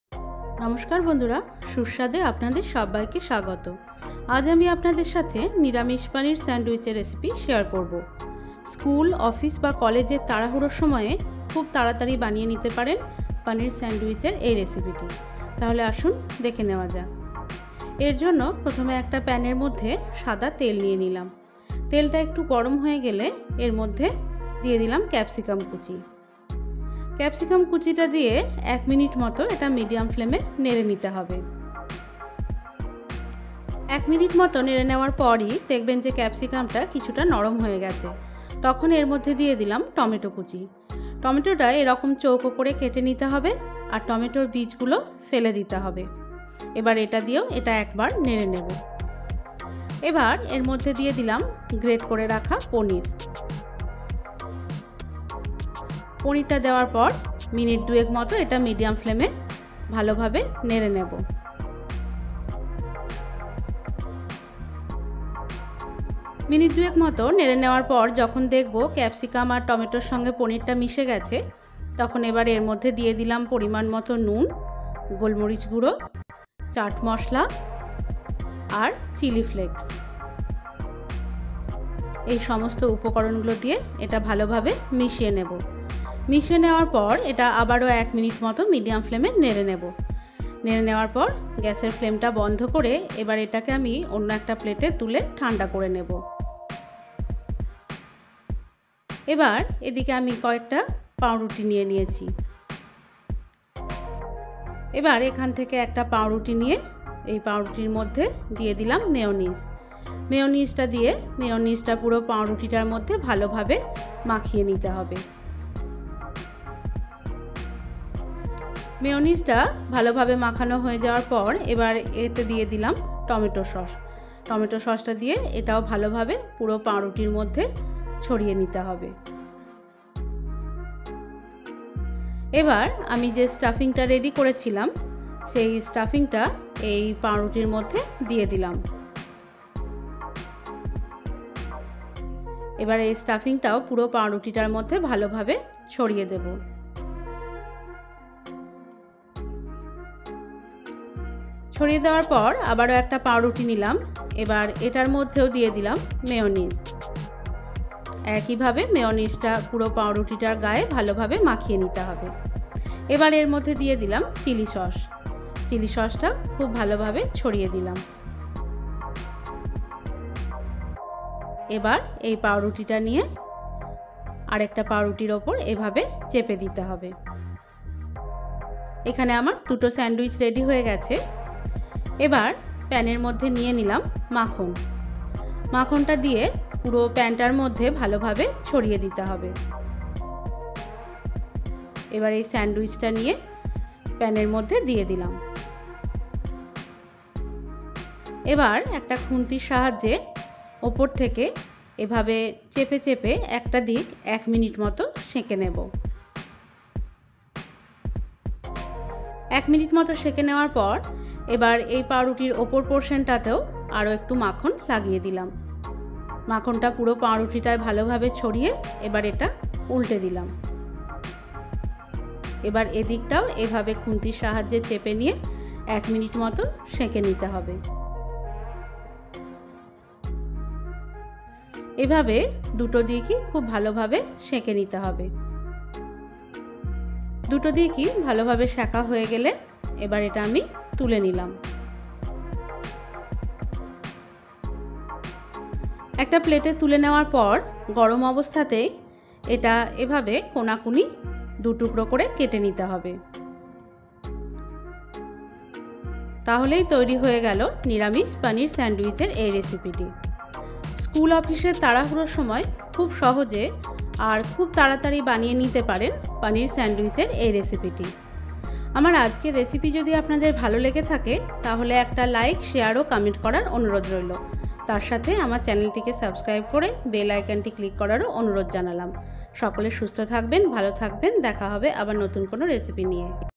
Paneer Sandwich Without Onion & Garlic|নিরামিষ পনির স্যান্ডউইচ স্কুল, অফিস, কলেজের টিফিনের জন্য
Ingredients (for 4 sandwiches)

150 gm paneer
8 pieces bread
1 /2 capsicum
1 tomato
1 /2 tea spoon black pepper powder
1/2 tea spoon chat masala powder
1/4 tbsp chilli flakes
salt ( as per taste)
4 tbsp mayonnaise
2 tbsp tomato sauce
2 tbsp chilli sauce
1 tbsp refined oil

Hi friends!!
I came back with another new recipe- ''Paneer Sandwich ''.
I hope you all liked it & keep supporting me.🙂